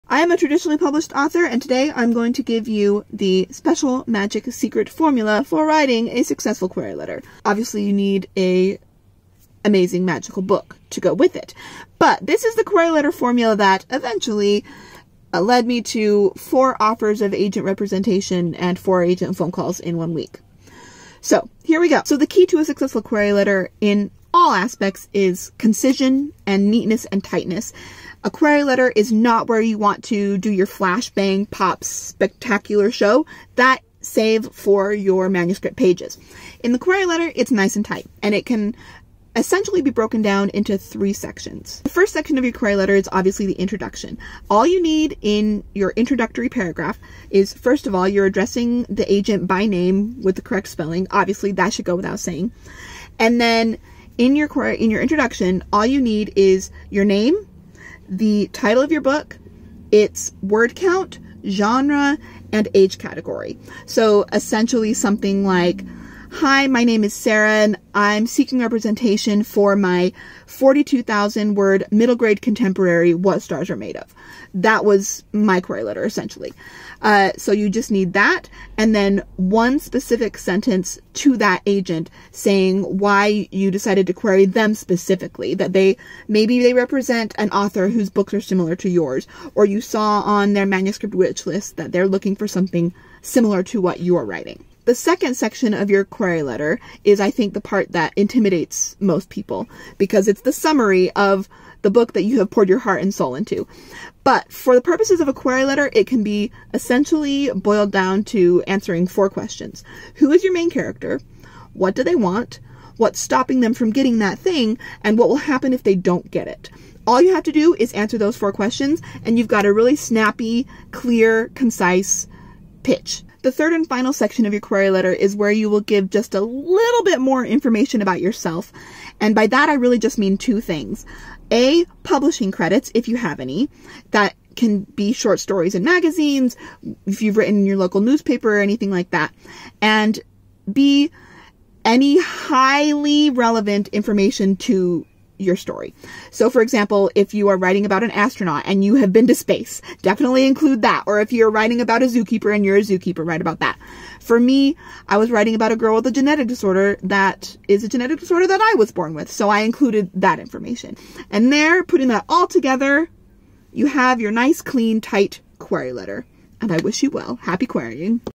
How To Write A Query Letter That Hooks A Literary Agent
I'm going to give you the special secret formula for how to write a successful query letter that hooks a literary agent. You've worked hard on your book, this is how to send it out in it's best light.

For more writing and publishing tips, sign up for my Substack

For more info on me and my books, check out my